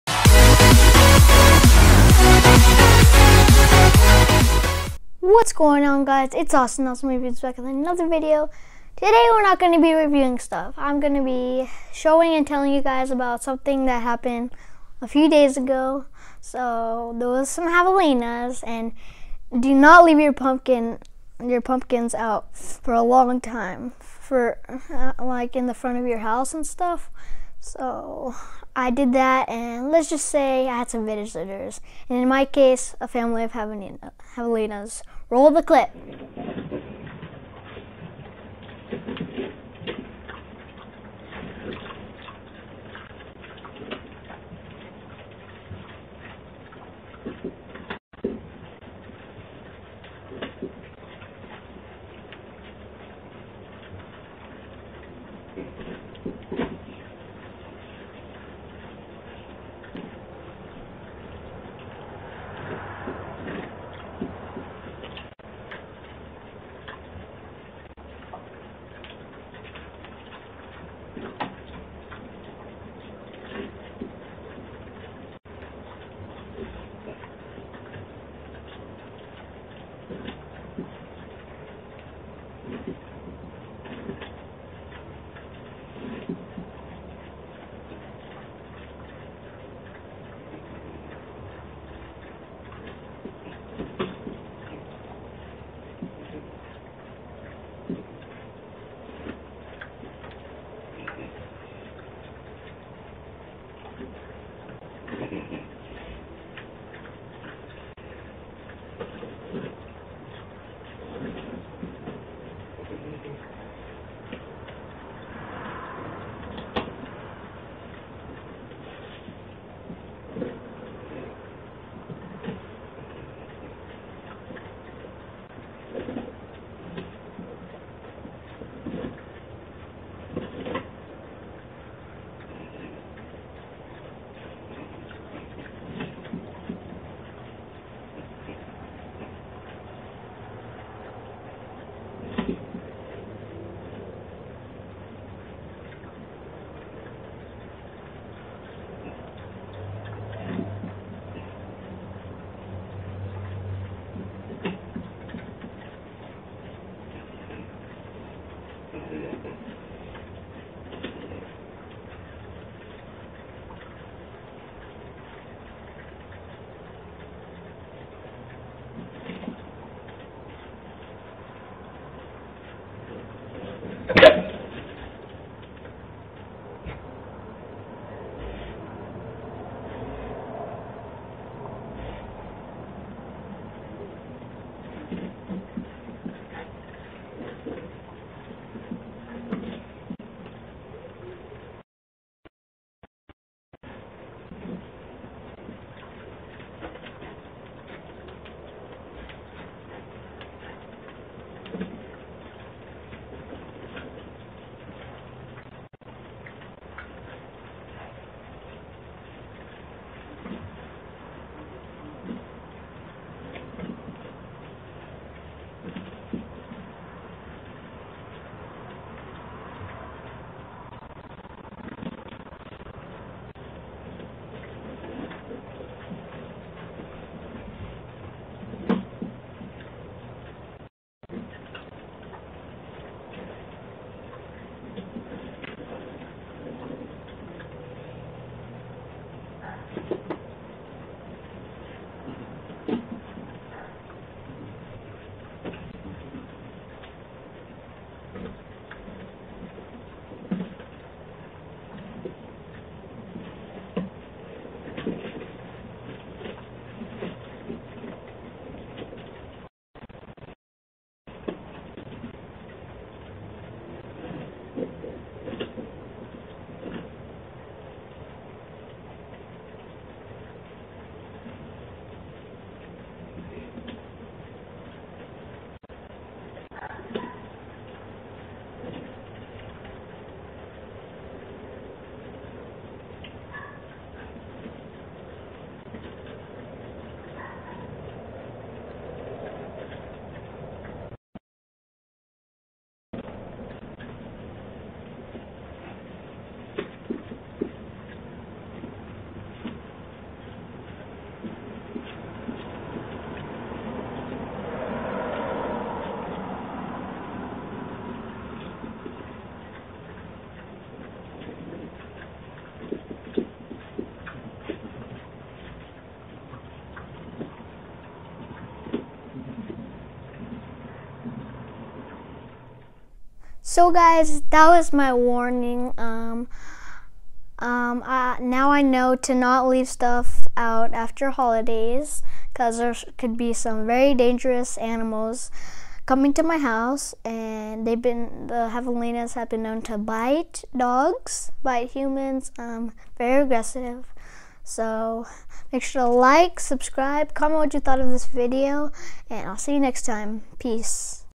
JAVELINA AT MY FRONT DOOR! Warning! DO NOT FEED JAVELINA!
WILD JAVELINA AT MY FRONT DOOR! Never leave out pumpkins from Halloween on your porch or this can happen. Javelinas are very aggressive and have attacked dogs and humans causing severe damage.